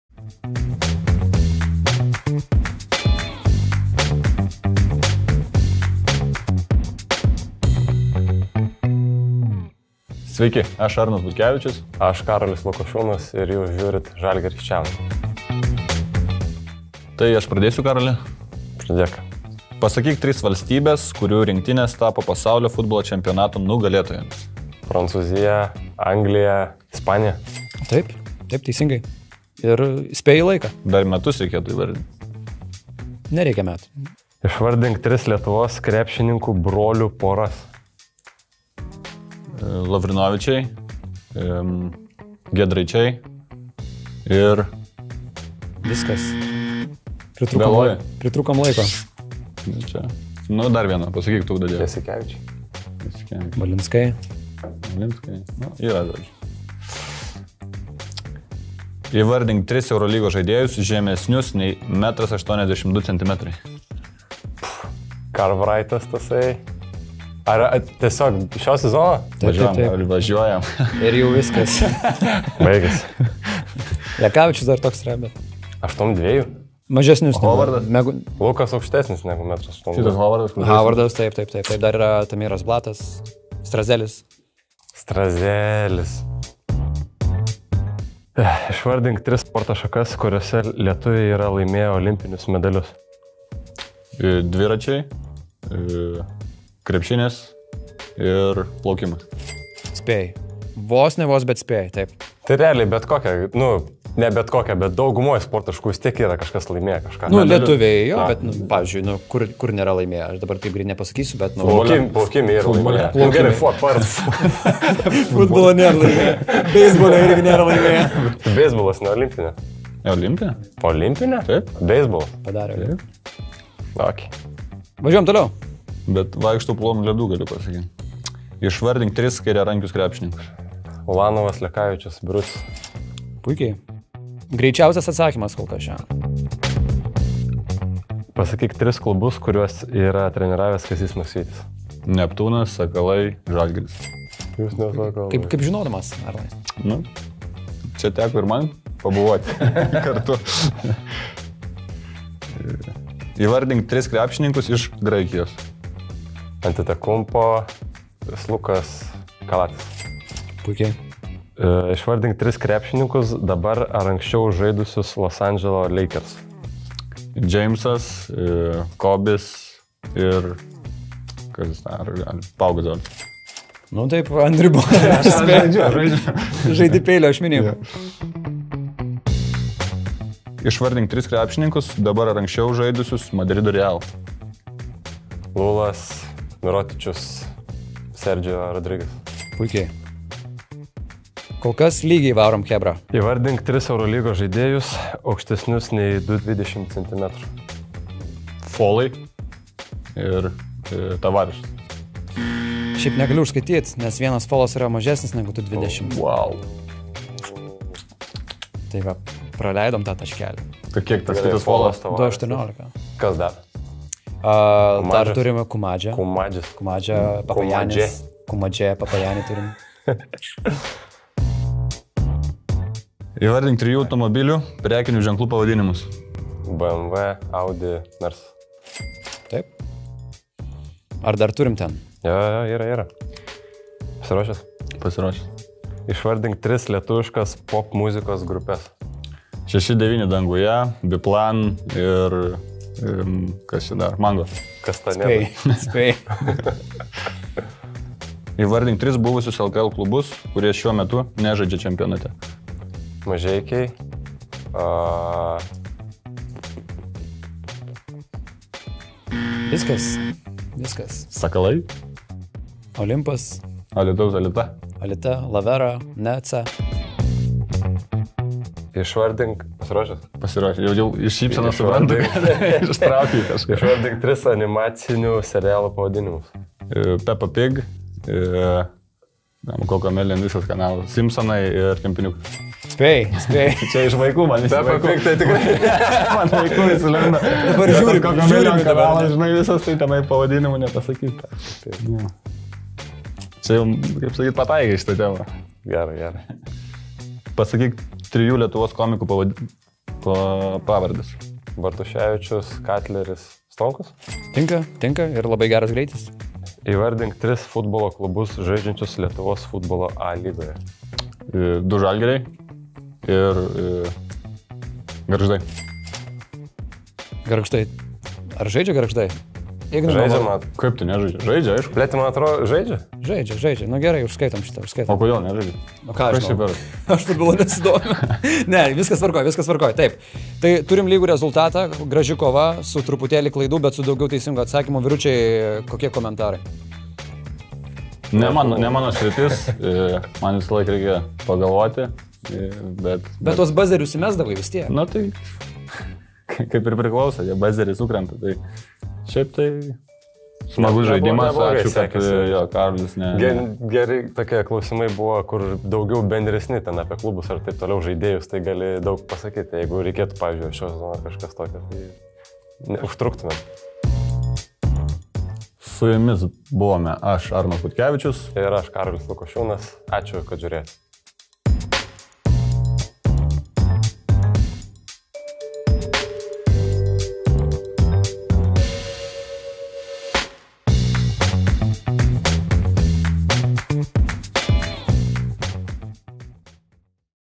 ŽalgirisChallenge: Lukošiūnas vs Butkevičius (7 Seconds Edition)
Žalgiriečiai Karolis Lukošiūnas ir Arnas Butkevičius susigrūmė ŽalgirisChallenge 7 sekundžių iššūkyje! Pasižiūrėkite, kuriam žalgiriečiui sekėsi geriau išnaudoti laiką ir pademonstruoti savo žinias.